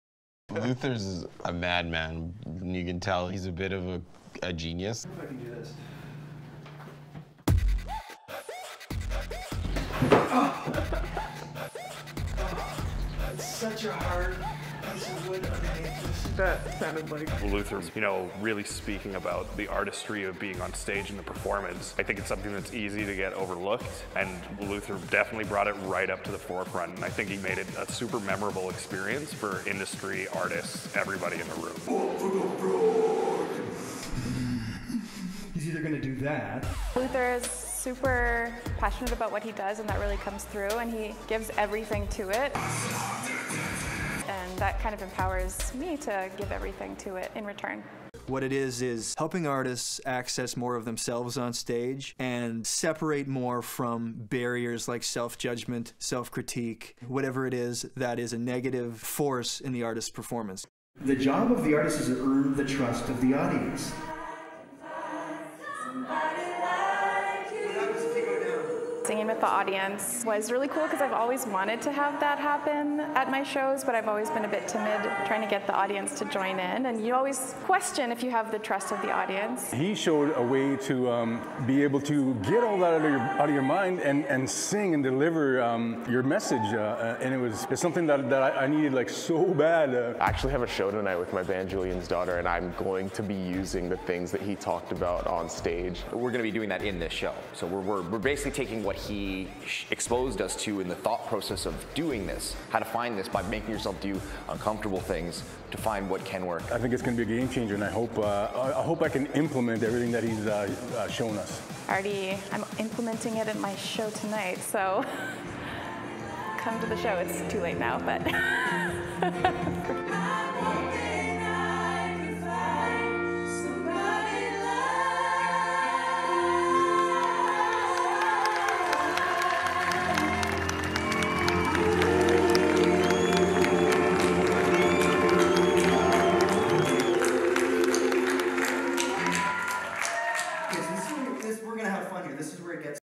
Destroy The Stage Keynote Trailer
A live performance coaching experience for artists like you've never seen.

In the past, performance coaching events have been banquet hall seminars with a stage, and a performer, and a coach telling them when to jump-kick to make their show razzle dazzle..

Destroy The Stage is not about choreography.

Almost every performer gets trapped in overthinking and obsessive judgment on stage, losing the connection to the art and the audience.

This problem is worse than ever now. Since the pandemic, artist mental health has truly suffered.

DTS is Luther Mallory's wild-energy, transformational, 2-hour performance coaching show focused on helping artists overcome the mental, emotional, and spiritual barriers that cause a loss of connection.

Controling the mind and staying connected is a learnable skill. We've witnessed hundreds of artists over a decade of coaching root into moment, let go of fear and judgment, risk more, and reconnect in an instant using the methods artists will learn at DTS.

"It's a performance coaching workshop for artists that feels more like a concert than a lecture," says Luther. "That's why we do it in a local venue- it feels like a show. I mean, people can usually drink at a DTS show. It's entertaining, funny, and wildly relevant to the life and experience of any artist. And we're challenging performers to risk more by letting go of overthinking and obsessive judgment.

Artists focus on learning marketing but performance is the foundation skill. It's how an artist connects with the audience that turns into the fanbase that gives an artist a career."

Audience participation is at the center of this show. We don't cherry-pick an artist to workshop on stage. We work spontaneously with anyone in the audience who wants to accept a challenge. We pull artists up on stage. We work with artists down in the crowd. And most of all, the entire audience works together to help each other let go and level up.

We want each artist to leave with new skills, not just the artists that take a challenge or come on stage to perform.

Curious what it's all about? Reach out at the link to find out more about how Destroy The Stage can elevate your community of artists and performers.

and get in touch.

Footage by:
BNB Studios, Winnipeg
Phil Kluba, Toronto
JUNO Masterclass, Toronto
CMI's AE Program, Toronto